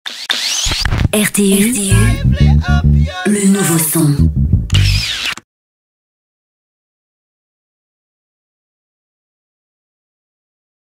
RTU jingle
Pour tous les sentimentaux, voici les anciens sons de cloches de la vaillante radio lyonnaise transformée désormais en super héroïne Nova Lyon.